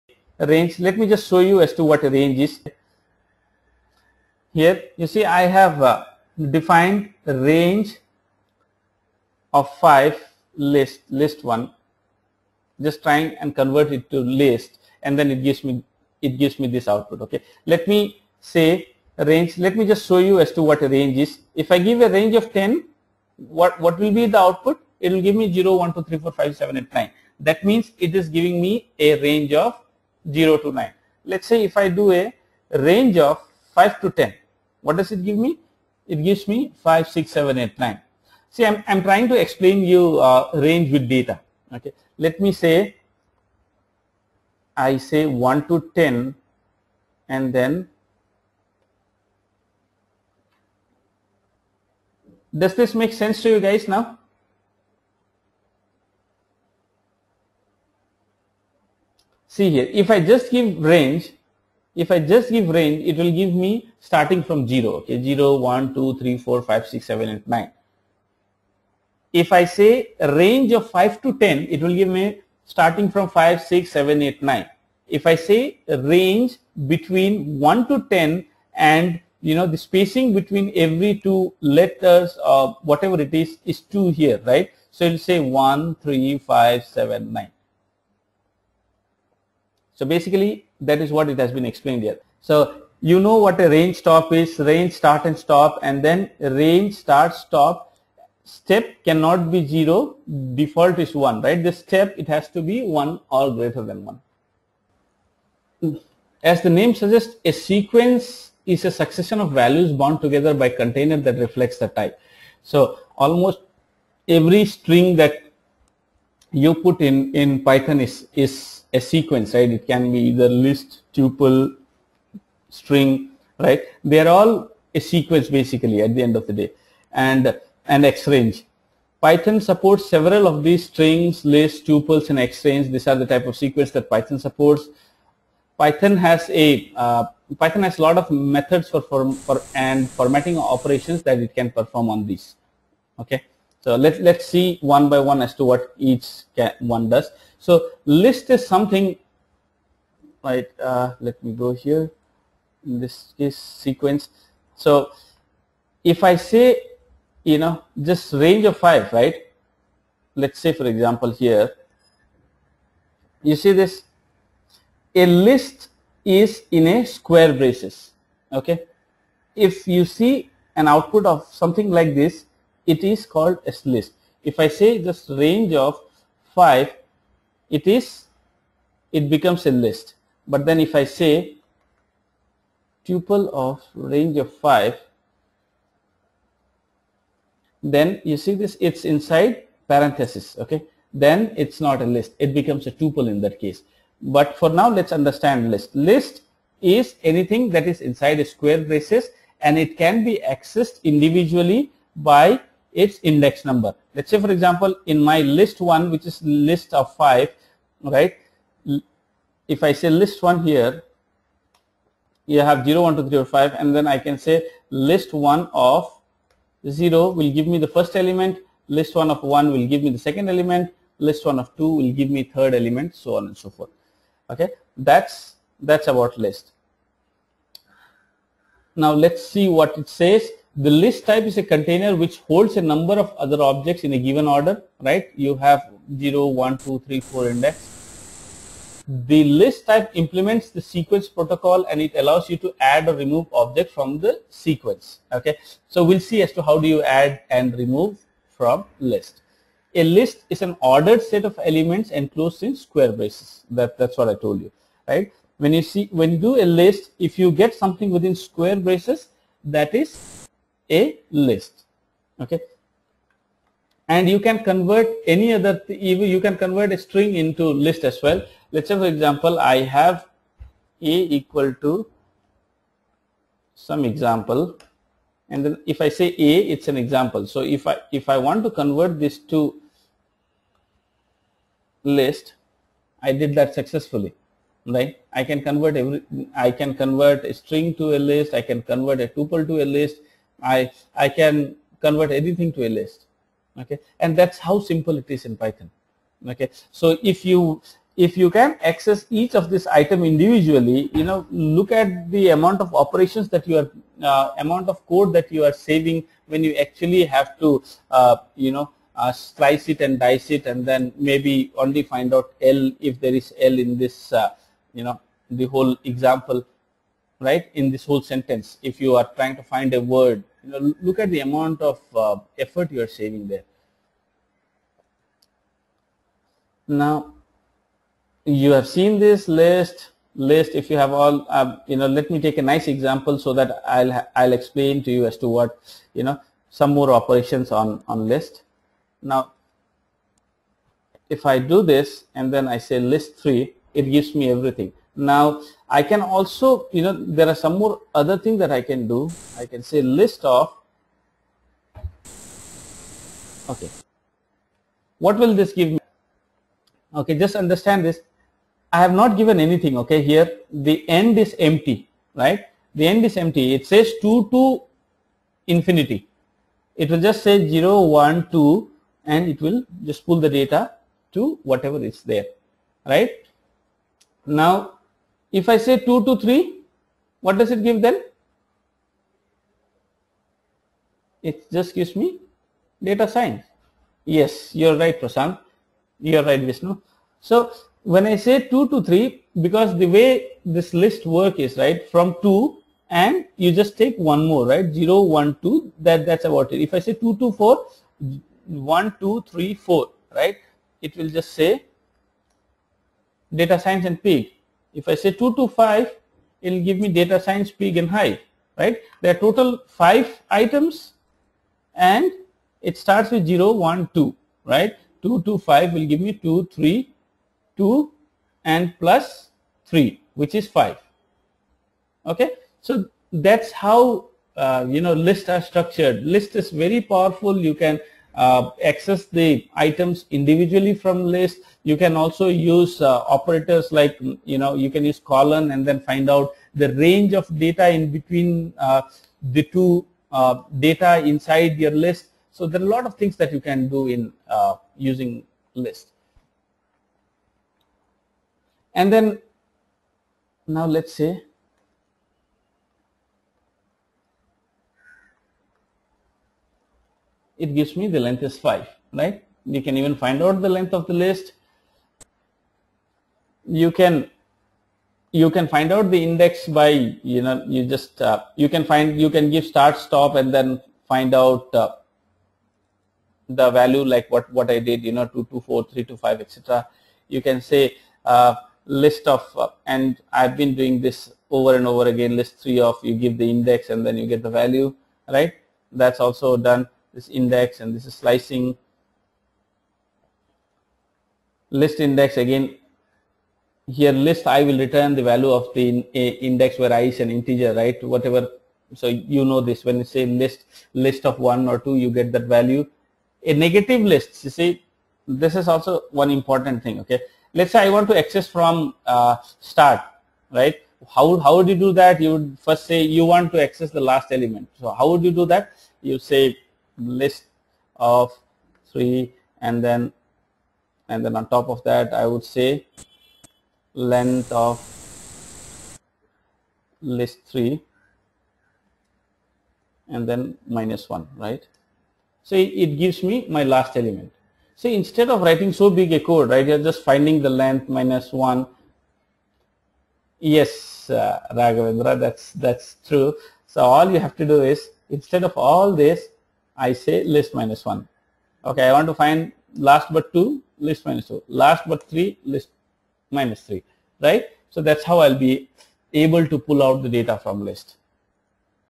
Understanding Range function and Sequences | Edureka
In Python, the built-in range function is very useful in generating the sequences of numbers in the form of list. The function, range() generates lists containing arithmetic progression. A sequence is a succession of values bound together by a container that reflects their type. Go through the entire video to understand those concepts. The primary topics covered are:
1.Range Function
2.Sequence
3.List
4. List- Example
5. List- Access
6. List- Indexing

The topics related to ‘Range function and Sequences’ have extensively been covered in our course ‘Python for Big Data Analytics’.